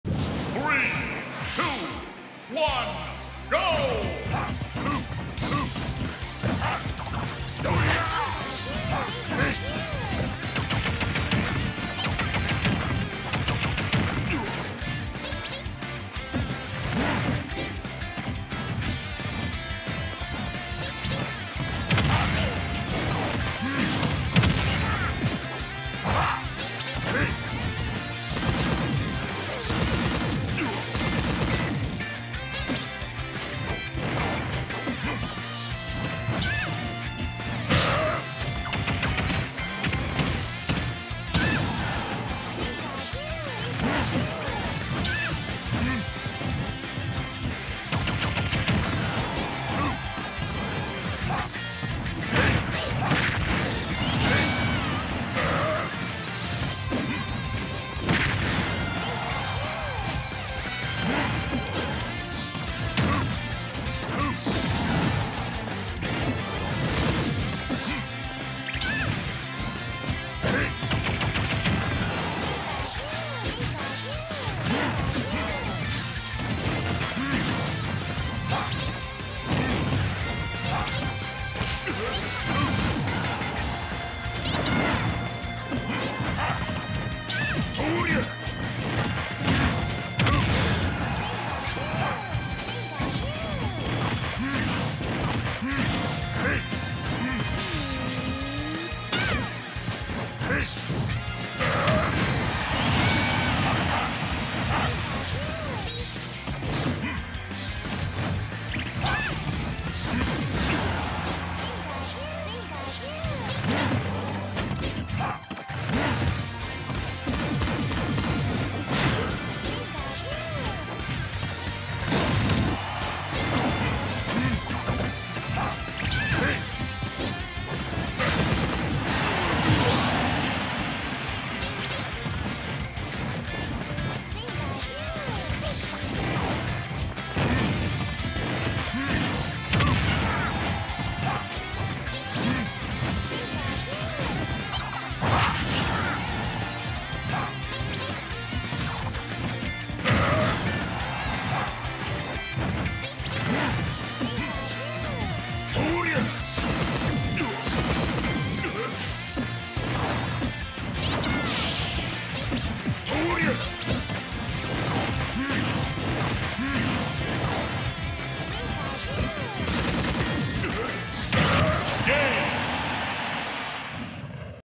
Standing still at death %
Honestly, when I got the Down Throw to Bair, I thought the opponent went AFK but then they continued to play the match normally. I think this was a moment of raw randomness that didn't pay off.